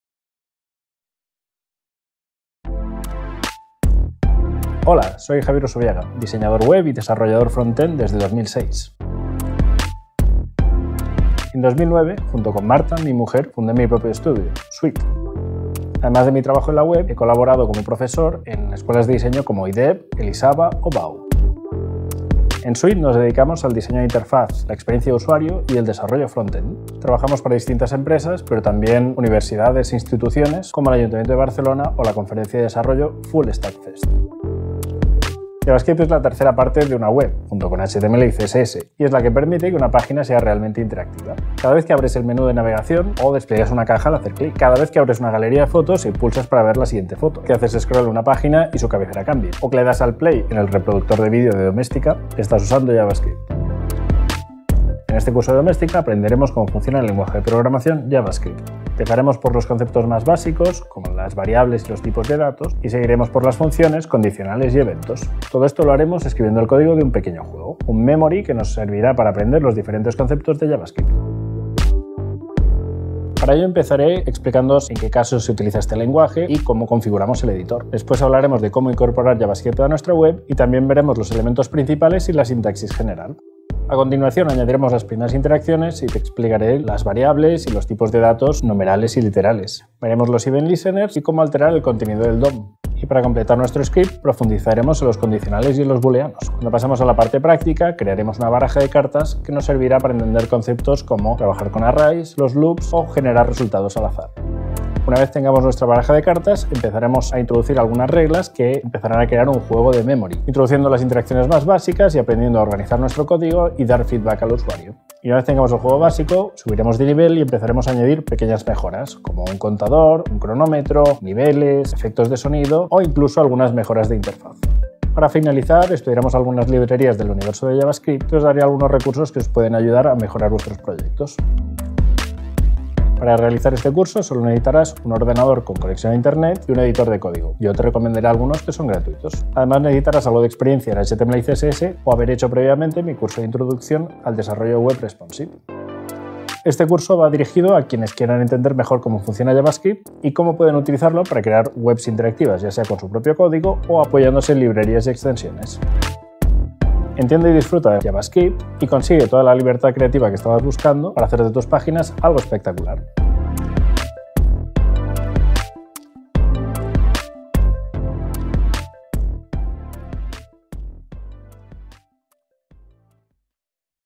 Curso online - Introducción a la programación en JavaScript - Domestika
En el curso online "Introducción a la programación en JavaScript", Javier Usobiaga, te enseñará el lenguaje de programación esencial para la creación de webs interactivas. Ver curso

Este curso es una introducción a JavaScript, partiendo desde lo más básico —sintaxis y variables— y progresivamente irás ampliando conocimientos —condicionales, arrays, loops, eventos—utilizando un juego de Memory que irás mejorando poco a poco de la mano de Javier Usobiaga, diseñador web y fundador del estudio Swwweet con más de 10 años de experiencia.

Si este es tu primer acercamiento al diseño web, es recomendable que hagas antes el curso, también de Javier Usobiaga, de Introducción al Desarrollo Web Responsive con HTML y CSS.